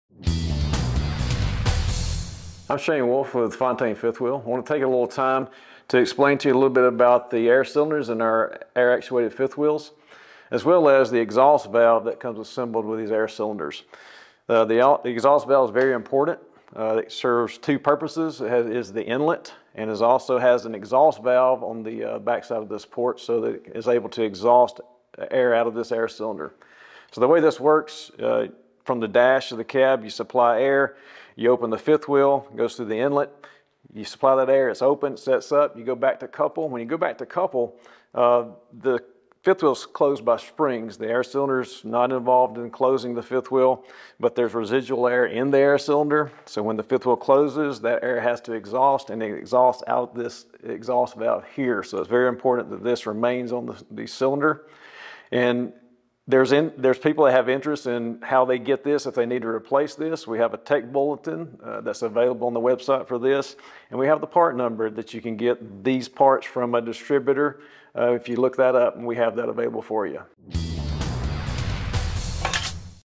Fifth Wheel Air Cylinder Exhaust Valve - Air Actuated
Learn about how to buy the Air Cylinder Exhaust Valve for your Fontaine fifth wheel in the aftermarket.

No-Slack® KIT-AA-6000L currently includes the JEV-F4M4 valve.
However, sometimes you may need to replace just the valve. Fontaine currently uses Clippard JEV-F4M4. This part and others like it are readily available at multiple parts distribution stores.